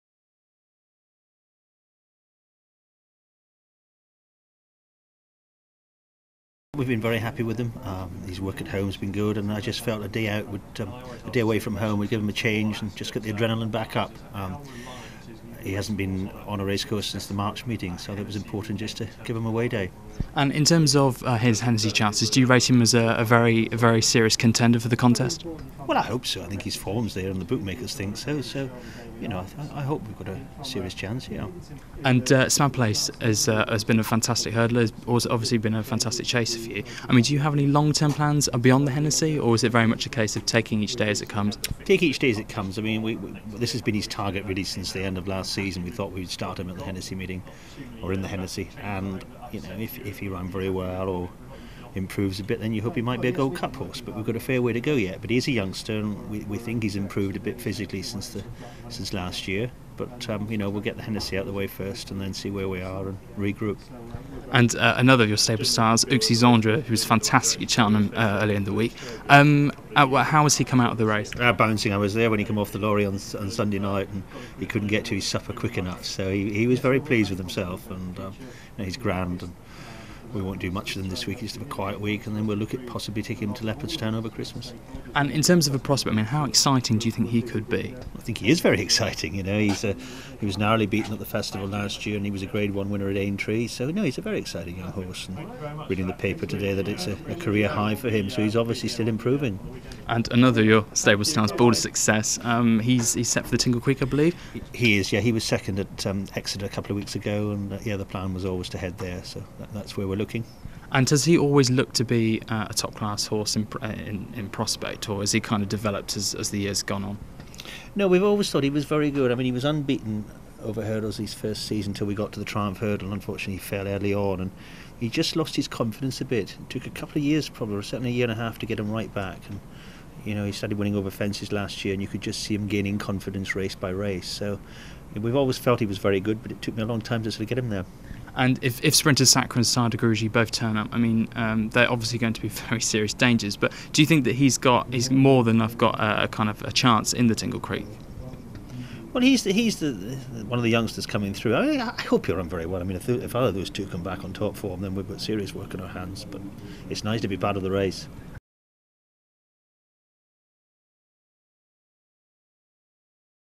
Alan King's Hennessy hopes and stable stars
Top trainer Alan King spoke to us about his main Hennessy Gold Cup hope Smad Place, who could turn into a Cheltenham Gold Cup contender, as well as updates on stable stars Uxizandre and Balder Succes.